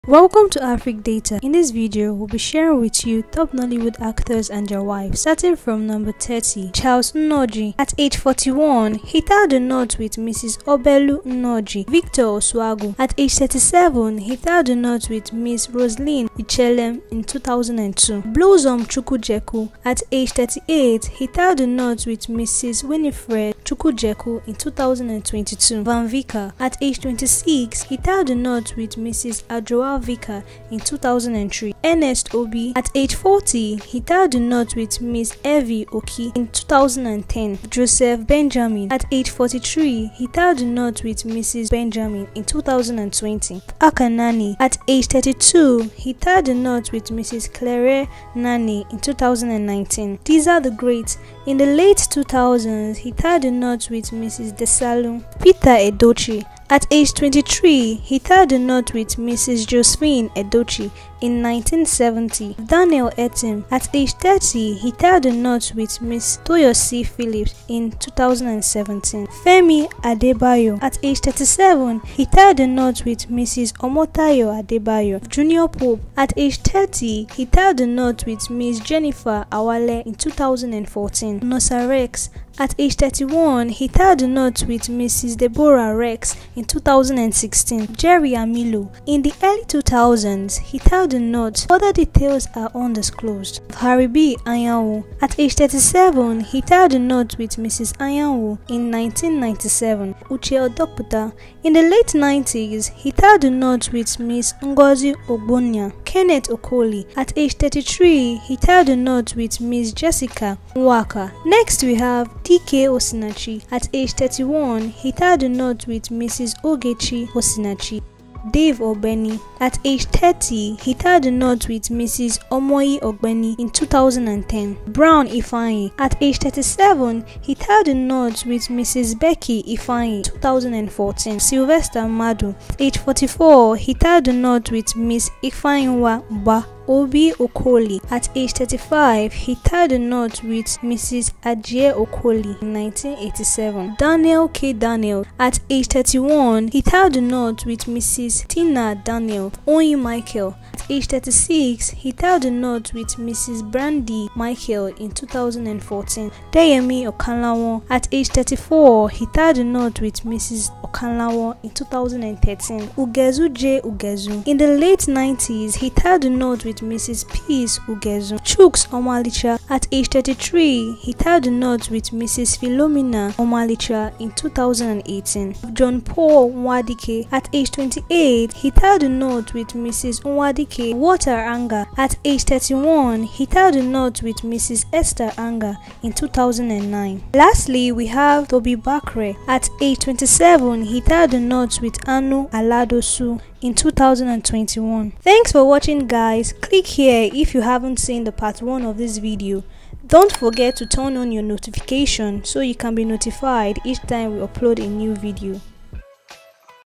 Top 30 Nollywood Actors & their Wives You probably haven’t seen before (Part 2)
I’m this video I shared a list of nollywood actors & their wives, date and age of marriage

Some of the these actors include

1. Charles inojie
2. Victor osuagwu
3. Blossom chukwu
4. Van vicker
5. Ernest obi
6. Joseph Benjamin
7. Akah nnani
8. Deza the great
9. Peter edochie
10. Daniel Etim
11. Femi Adebayo
12. Junior pope
13. Nose Rex
14. Jerry amilo
15. Harry banyanwu
16. Uche odoputa
17. Kenneth okonkwo
18. Dike osinachi
19. Dave ogbeni
20. Brown ifeanyi
21. Sylvester madu
22. Obi okoli
23. Daniel k Daniel
24. Onny micheal
25. Deyemi okanlawon
26. Ugezu j Ugezu
27. Chuks omalicha
28. John paul nwadike
29. Walter anger
30. Tobi bakre